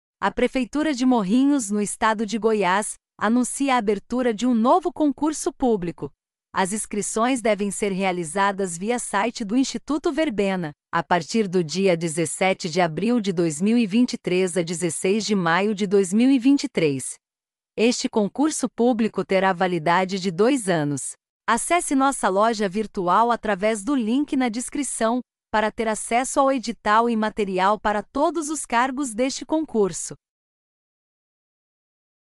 Apostila PREFEITURA DE MORRINHOS GO 2023 Monitor Educacional
A Prefeitura de Morrinhos, no estado de Goiás, abre Concurso Público que tem objetivo de preencher 1.329 vagas e formar cadastro reserva em inúmeros cargos.

As inscrições devem ser realizadas via site do Instituto Verbena, a partir do dia 17 de abril de 2023 a 16 de maio de 2023, até as 17h, com a taxa sendo de R$ 50,00 a R$ 160,00.

➔ Cadastre-se para receber atualizações de todos os concursos 2023. Concurso PREFEITURA DE MORRINHOS GO 2023 👇